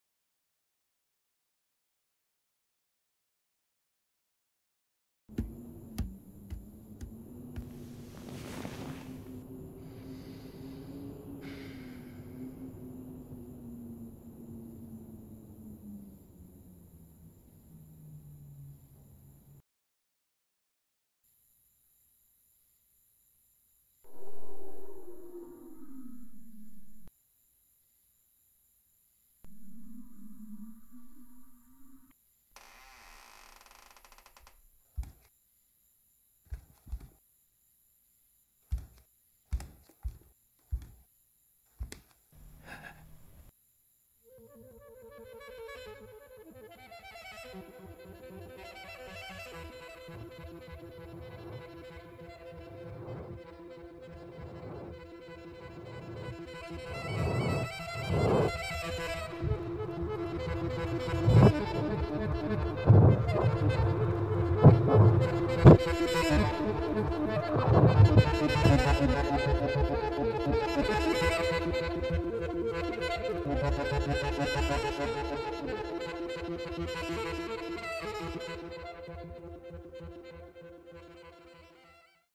"Lumberwood" | ANIMATIC
This is my animatic made during pre-production development of my current project. I wanted to play with sound design and creating a sense of atmosphere in the scene for how short it is.